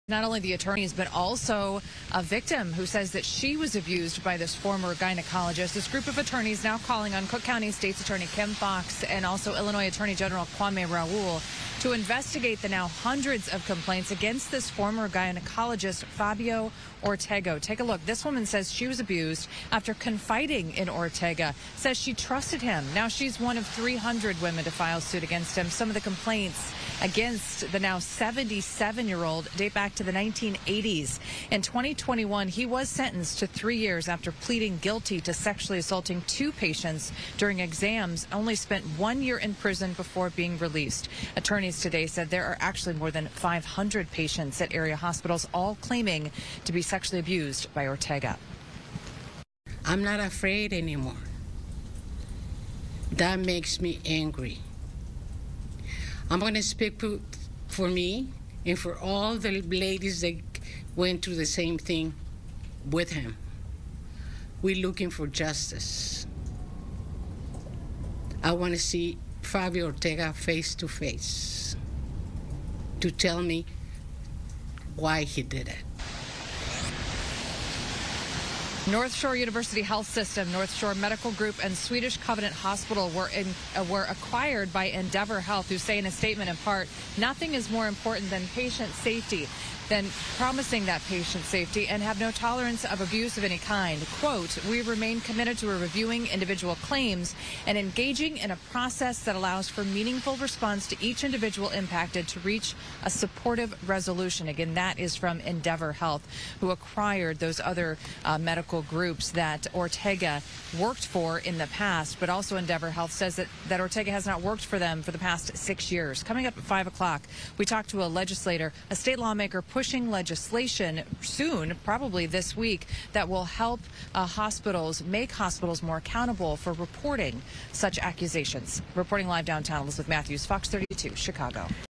Former Chicago gynecologist faces hundreds of abuse allegations, calls for reopened investigation
Many are calling to reopen an investigation into a local doctor who is facing hundreds of allegations of patient abuse. Elizabeth Matthews has the story.